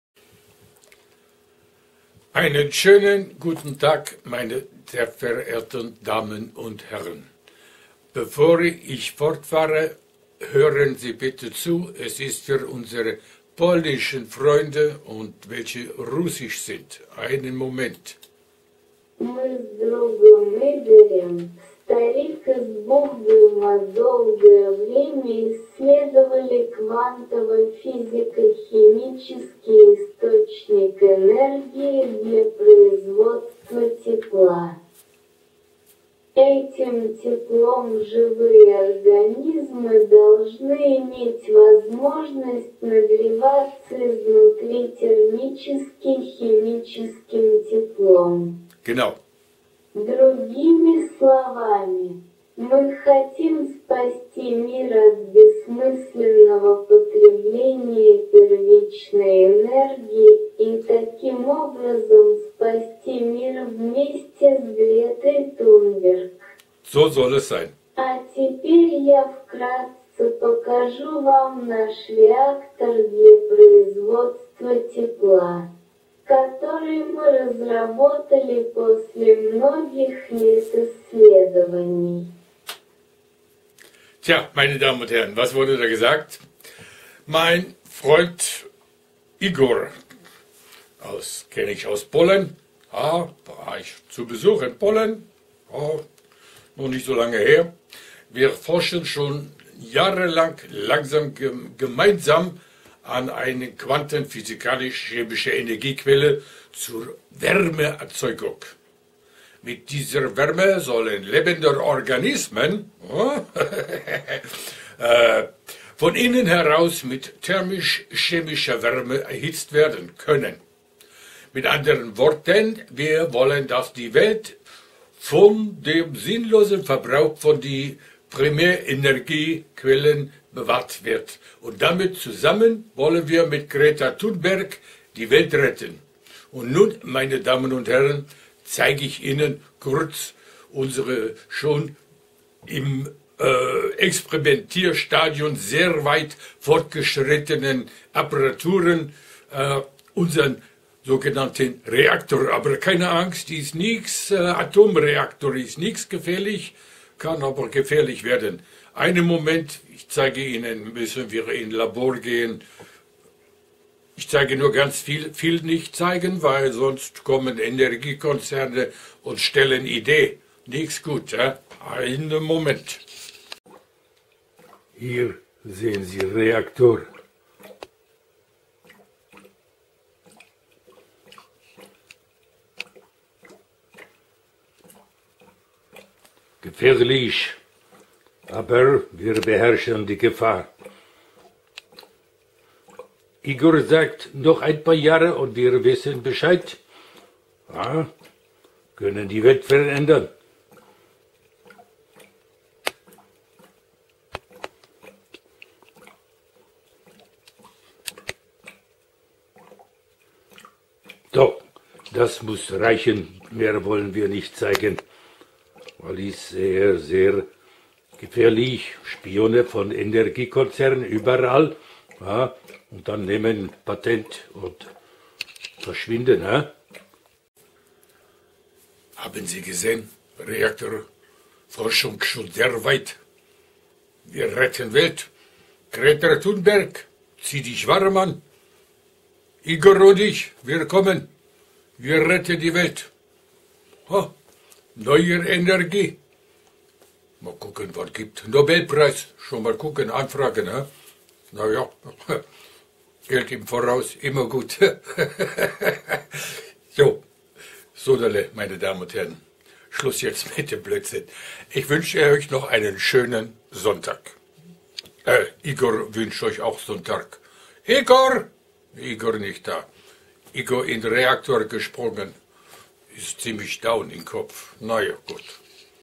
Igor und Ich, wir retten die Welt
Tscha, meine Damen und Herren, nach jahrelanger Forschung mit vielen Rückschlägen ist es uns, Igor und mir gelungen einen kleinen Fortschritt in Sachen Energiegewinnung mittels Biochemischer Prozesse in Gang zu setzten, welcher in der Lage ist unter Einbeziehung der Quantenforschung
im Bereich der sogenannten Freien Energie, welche auch als dunkle Materie vermutet wird, welche durch Zweisteins Relativitätstheorie gestützt wird und nichts mit den schwarzen Löchern im Kopf einiger Weltverbesserer zu tun hat. Natürlich werden wir, Igor ( der schon mit Dr. Frankenstein zusammen gearbeitet hat ) und ich, unsere bisherigen Forschungsarbeiten geheim halten, um eine Eliminierung von Seiten der Erdöllobby zu entgehen.